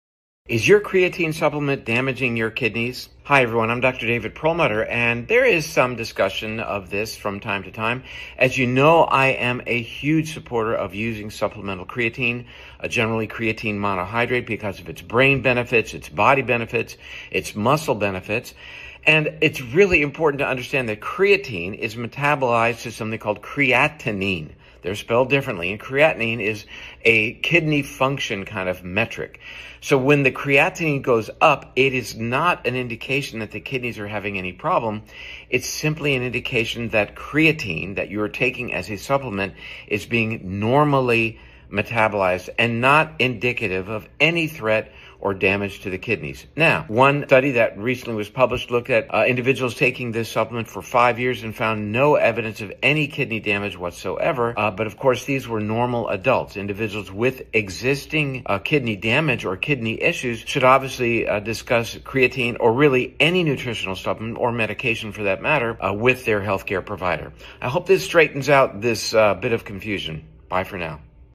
Is Creatine Safe for Your Kidneys?
I’ve been talking a lot about creatine lately, and one concern comes up again and again: kidney health.

Many people worry that creatine damages the kidneys or that higher creatinine on a lab test automatically means something is wrong. The reality is more nuanced. Creatine is naturally converted in the body to creatinine, and elevated levels can simply reflect normal metabolism rather than kidney disease.

Long-term research has added important clarity to this conversation. It’s worth understanding the science before drawing conclusions about your health or your supplements.